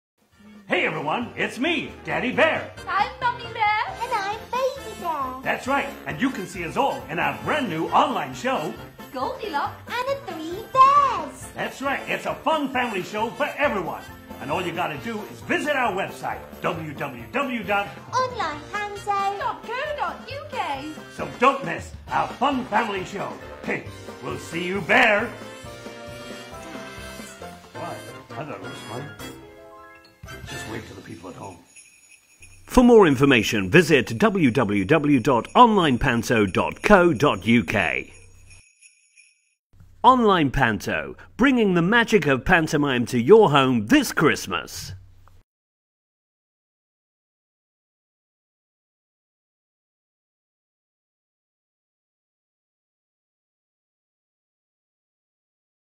Goldilocks advert
Streaming live this Christmas

Roll up! Roll Up!
This festive season let us bring panto to you! Enjoy this bright, colourful, festive and fun family show in the safety of your own social bubble! Featuring our heroine Goldilocks and her 3 new friends Mummy Bear, Daddy Bear and Baby Bear as they save Dame Tilly’s Circus of Wonders from the villainous and dastardly Creepflesh! This show is packed full of traditional comedy, slapstick and pantomime fun! So, don’t delay book now for the Pantomime that’s just right!